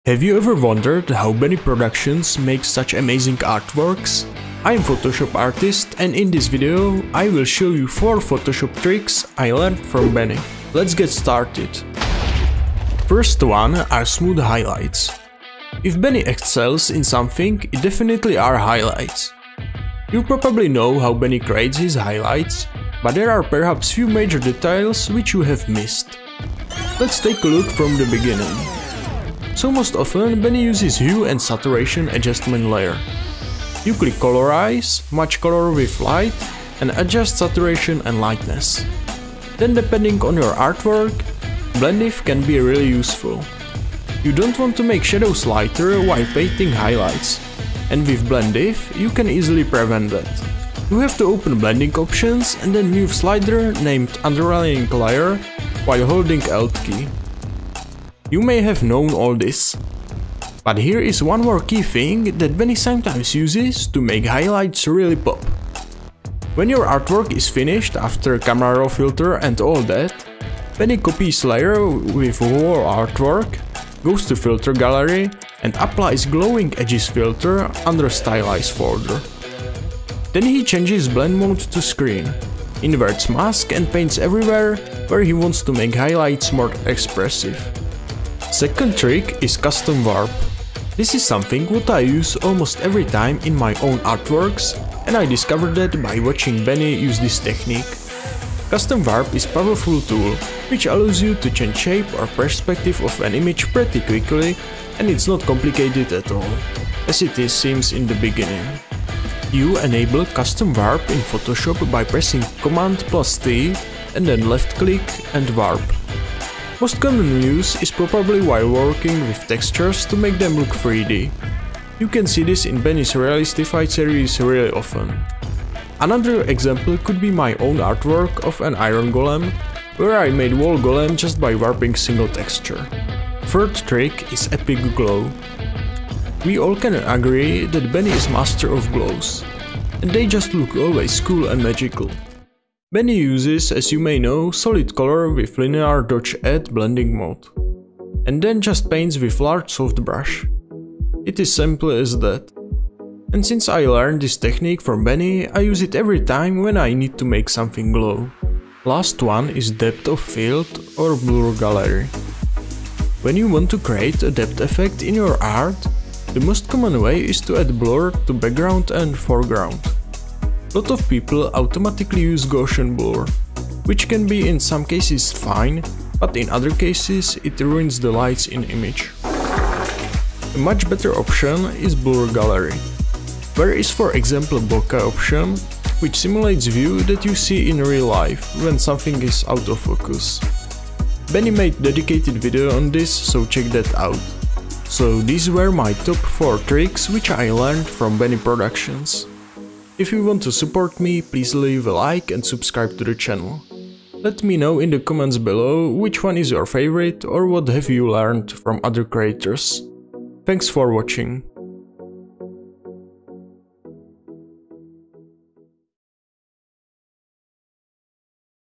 4 Secret Photoshop TRICKS I Stole from Benny Productions
I will show you my top 4 secret photoshop tricks and techniques that I learned from Benny Productions. This is not complete photoshop tutorial. You will see how to make highlights, custom warp and more as Benny Productions does in his realistified series.

Chapters:
0:00 - Intro
0:13 - Trick 1
1:28 - Trick 2
2:14 - Trick 3
2:39 - Trick 4
3:15 - Outro